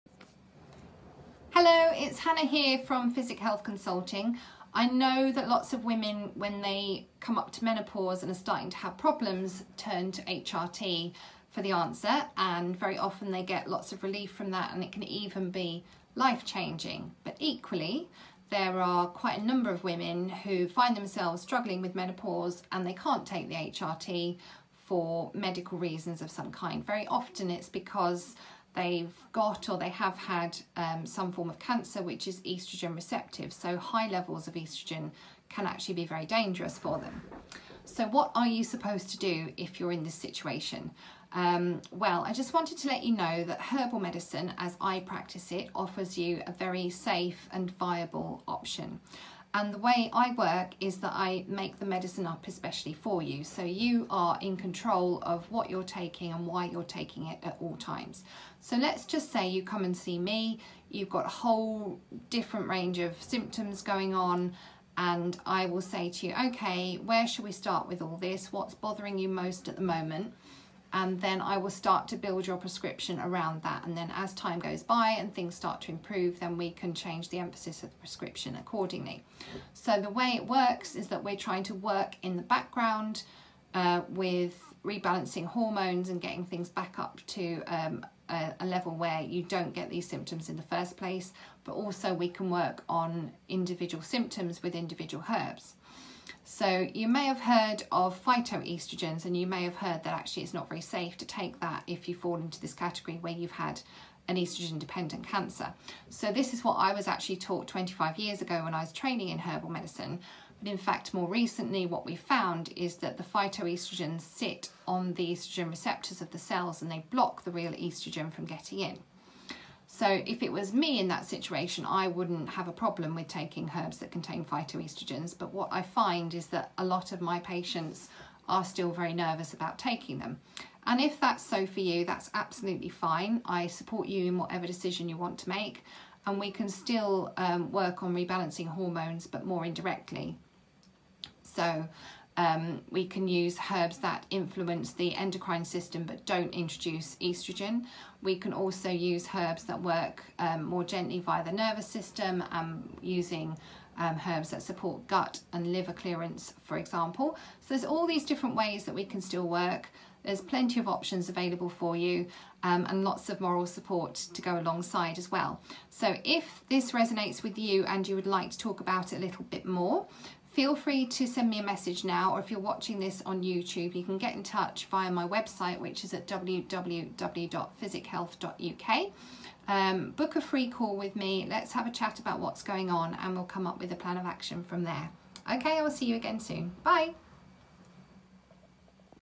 What To Do When You Can’t Take HRT
HRT (Hormone Replacement Therapy) is a lifeline for many women going through menopause, but others can't take it for medical reasons. Here are some options for you if you can't, or would prefer not to take HRT for any reason.